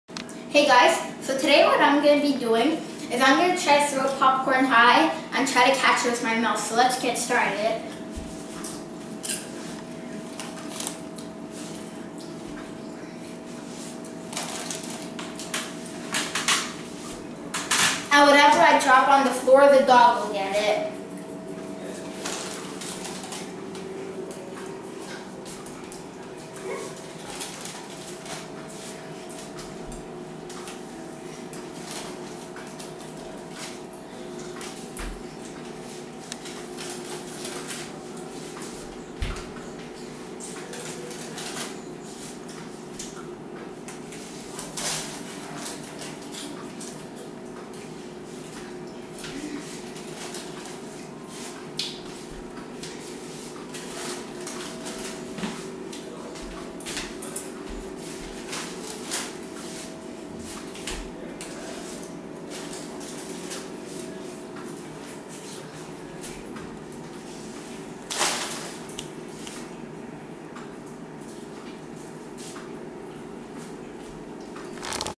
Trying To Eat Popcorn Differently
Throwing popcorn up in the air and trying to catch it with my mouth.